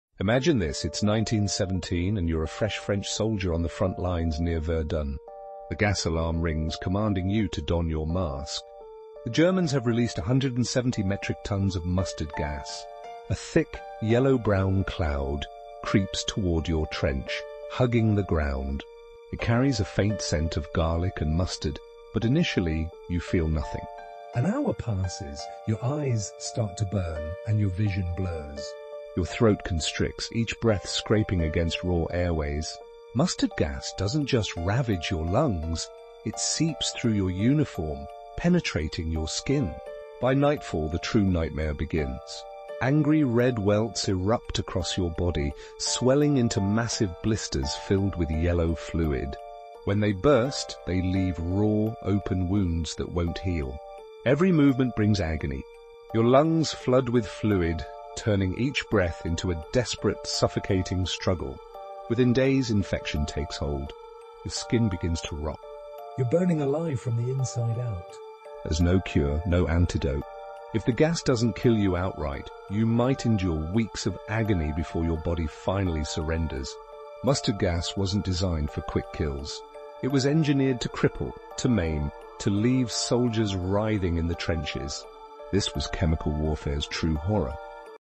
Surviving Mustard Gas: The Horrifying Effects of Chemical Warfare
The devastating effects of chemical warfare are a harsh reality of modern combat. Mustard gas, in particular, is a highly toxic chemical agent that can cause severe burns, blisters, and respiratory problems. In this video, we explore the horrifying effects of mustard gas on the human body and discuss the importance of combat medicine in treating its victims. We'll delve into the history of chemical warfare and examine the ethical dilemmas surrounding its use. How can soldiers and civilians alike survive the deadly consequences of chemical agents? What are the most effective methods of chemical resistance? Join us as we uncover the dark truth about surviving mustard gas and the importance of upholding warfare ethics in the face of such devastating weapons.